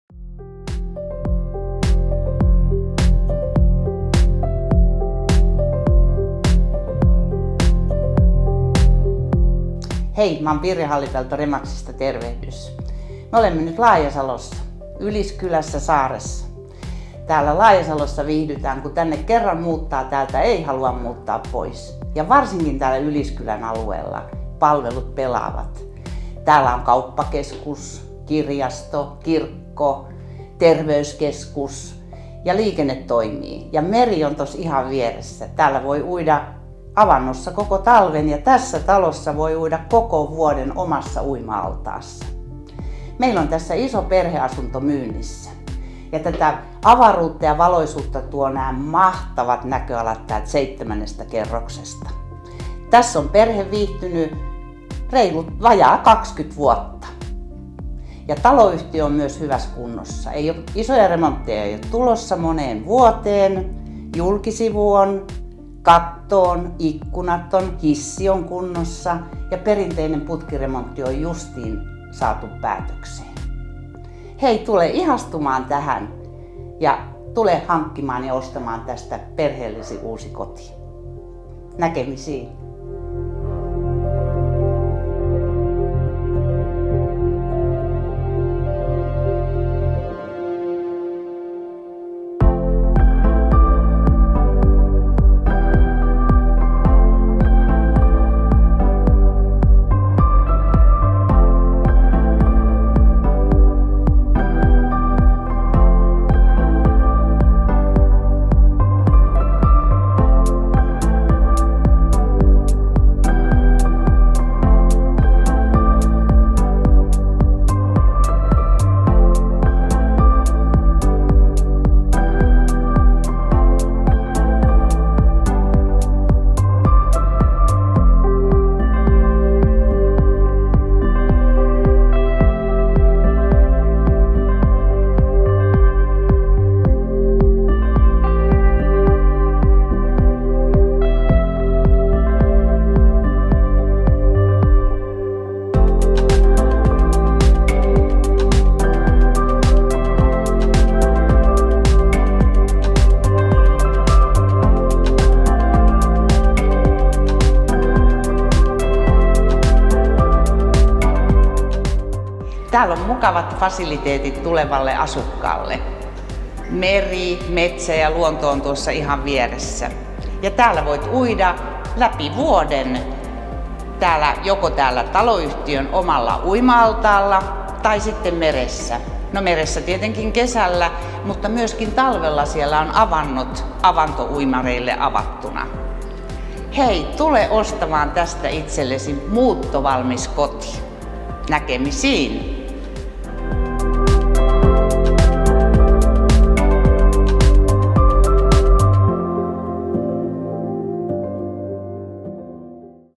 Marunakuja 4 perheasunto
Seitsemännen kerroksen neljän makuuhuoneen koti hyvin hoidetussa taloyhtiössä. Putket tehty 2022. Omakin sauna on huoneistossa, mutta myös taloyhtiössä on sauna ja uima-allas.